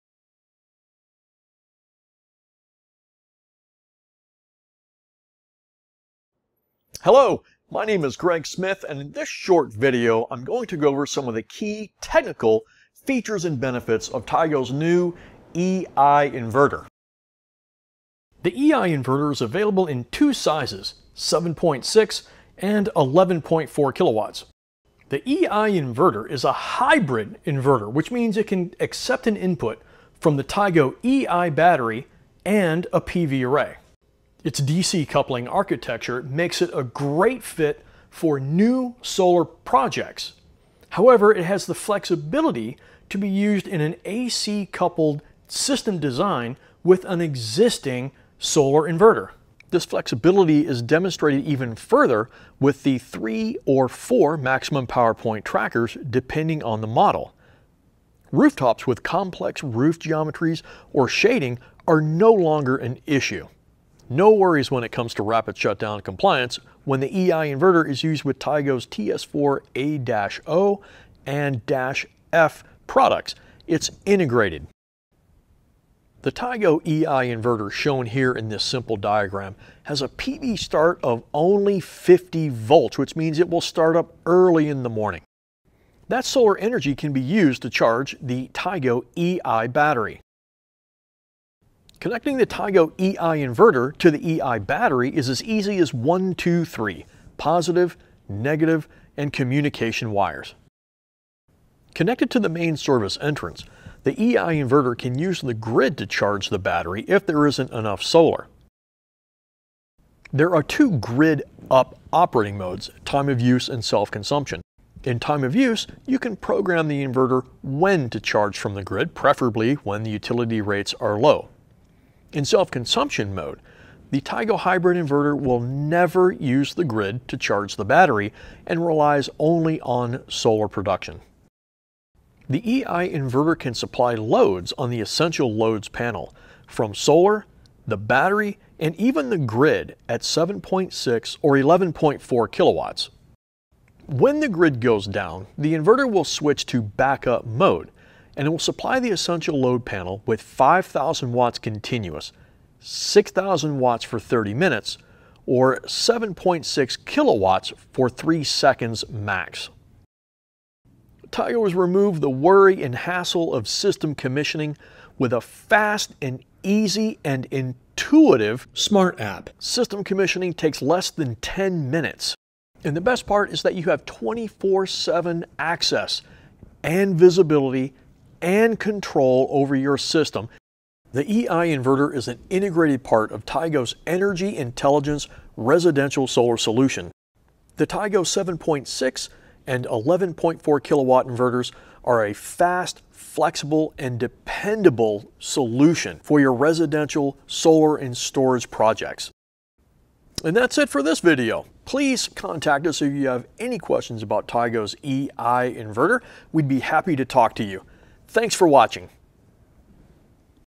Tigo EI Inverter Specifications Overview
The Tigo EI Inverter is the centerpiece of the Tigo EI Residential Solution. This video provides an overview of the specifications and technical information about the Tigo EI Inverter.

For more information on the EI Battery - including specification sheets, brochures, diagrams, and more